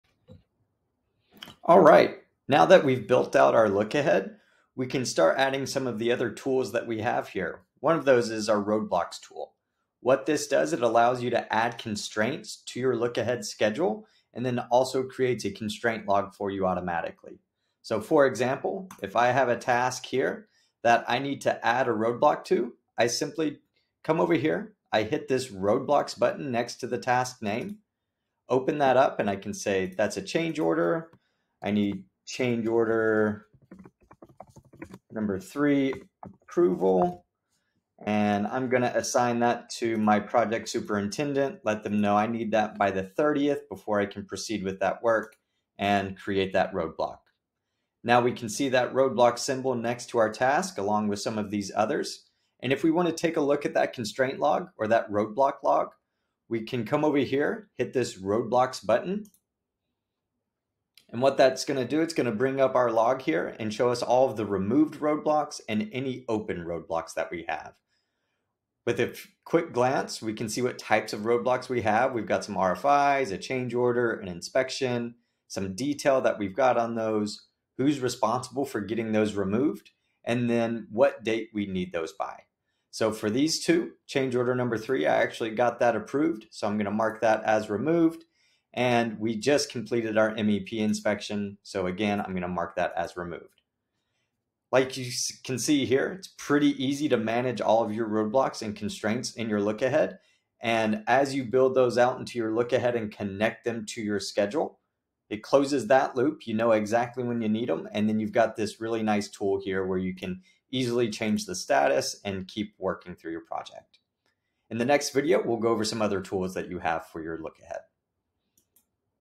How to create and update Roadblocks or Constraints in the Lookahead | Outbuild
Now that we built out our Lookahead we can start adding some of the others tool that we have in Outbuild, one of those is our Roadblock or Constraints tool. What this does, it allows you to add constraints to your Lookahead Schedule and then, it also create a Constraints Log for you automatically.